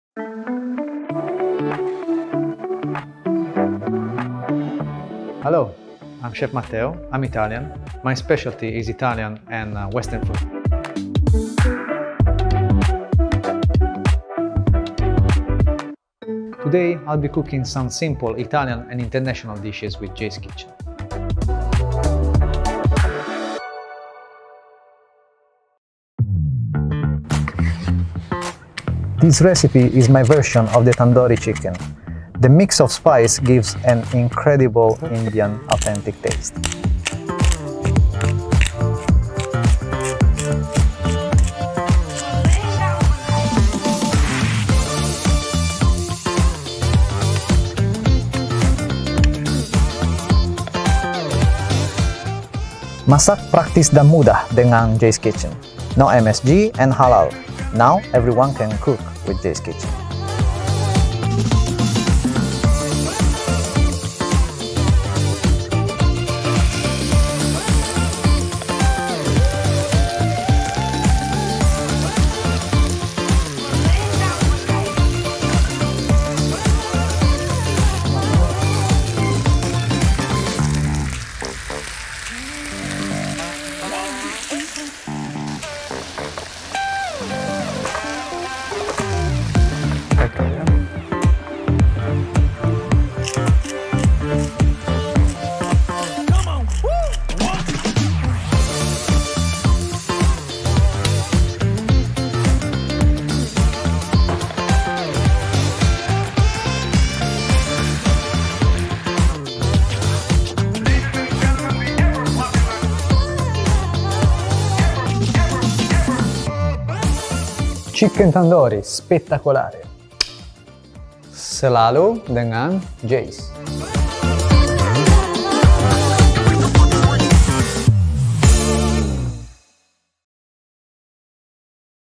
Resep Chiken Tandoori | Jay's Kitchen X Chef Matteo Meacci
Jay’s Kitchen dan Chef Matteo kembali menyambut hari mu! Kali ini Chef Matteo akan memasak makanan khas India yaitu Chicken Tandoori. Bikinnya cepat dan mudah loh, karena menggunakan Tandoori seasoning dari Jay’s Kitchen. Jadi nggak perlu repot-repot meracik bumbu deh. Ingin coba bikin juga? Yuk simak resepnya di bawah!

Bahan:
Dada Ayam, fillet 200 g
Bawang Bombay, cincang 40 g
Blackpepper Jay’s Kitchen
Yoghurt 50 g
Santan 50 g

Cara membuat:
1. Sayat melintang daging ayam agar bumbu bisa meresap masuk ke dalam.
2. Campurkan daging ayam dan bawang bombay dalam mangkuk dan masukkan Blackpepper dan Seasalt Jay’s Kitchen secukupnya.
3. Masukkan yoghurt, santan dan Tandoori Seasoning Jay’s Kitchen secukupnya kemudian aduk hingga merata
4. Marinasi dalam kulkas selama 30 menit
5. Setelah 30 menit, keluarkan dari kulkas dan panaskan minyak untuk menggoreng ayam
6. Goreng ayam hingga dua sisi permukannya kecoklatan. Setelah matang, tiriskan.
7. Dalam wajan yang sama, kita buat saus tandoori sebagai pelengkap. Masukkan santan, Jay’s Tandoori Seasoning secukupnya dan air secukupnya.
8. Koreksi rasa, tambahkan seasalt apabila diperlukan.
9. Masukkan ayam Tandoori ke dalam saus dan didihkan beberapa saat hingga mengental.
10. Ayam tandoori siap disajikan.